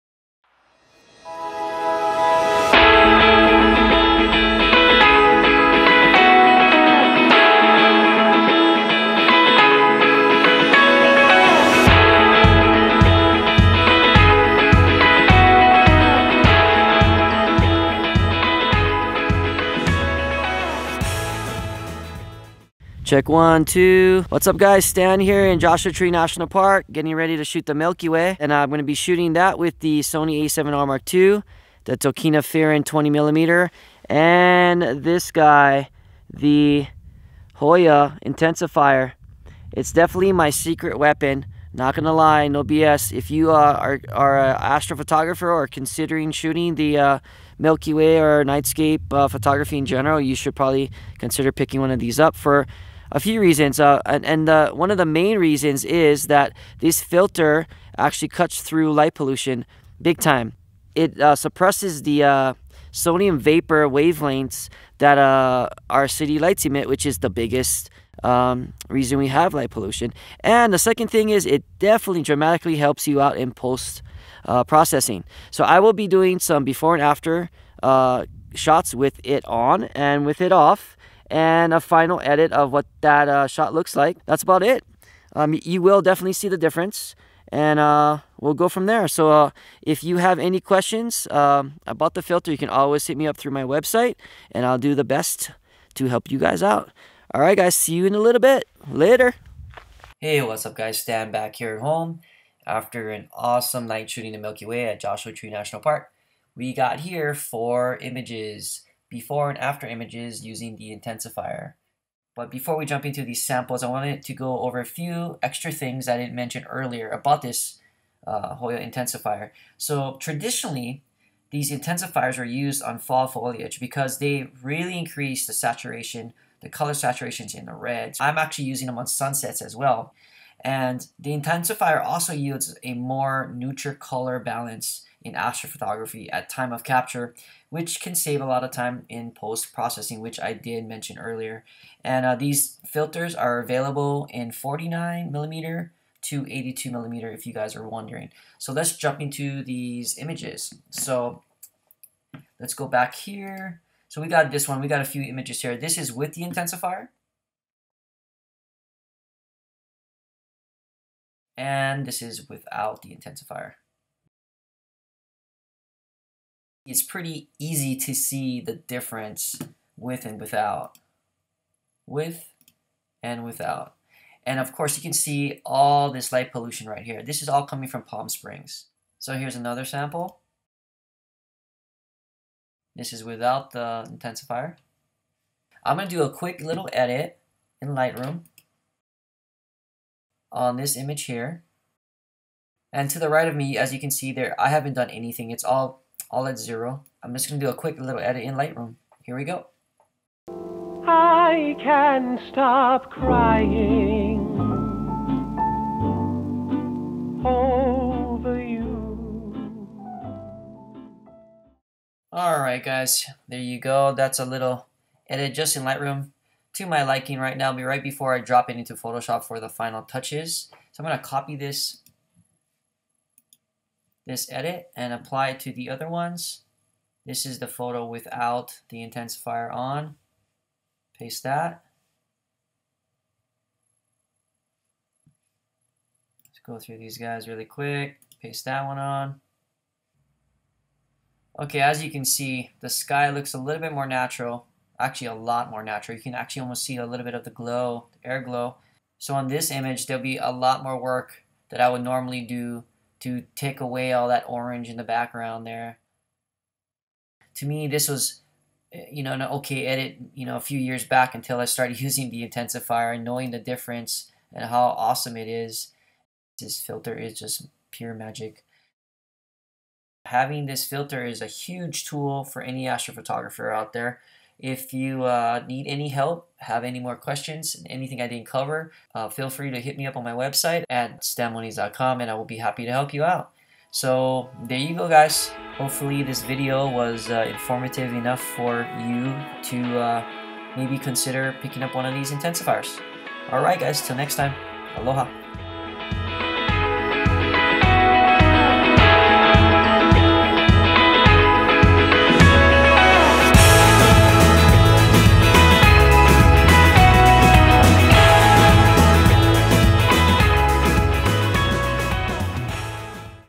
Tutorial: Cut light pollution like a knife
Hoya Filter USA ambassador Stan Moniz takes you out to Joshua Tree to capture some images and take you through his image processing to get the best final image possible.